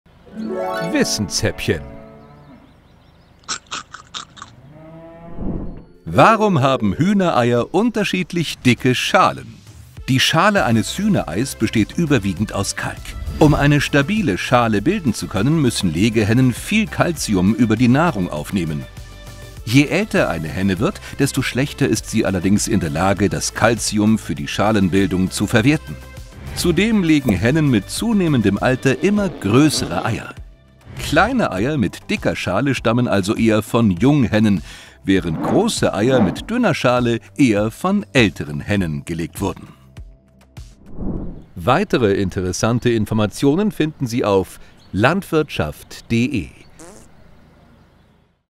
Warum haben Hühnereier unterschiedlich dicke Schalen?
Hühnereier unterscheiden sich nicht nur in Farbe und Größe voneinander, manche haben auch eine dickere Schale als andere. Woran liegt das?

Quellennachweise:
Intro:
Olga Kurbatova via Getty Images
PCH-Vector via Getty Images
Clip:
1 Nacho Mena via Getty Images
2  paad via Getty Images
3 BLE
4 boonkum via Getty Images
5 Vitaliia Hryshchenko via Getty Images
6 BLE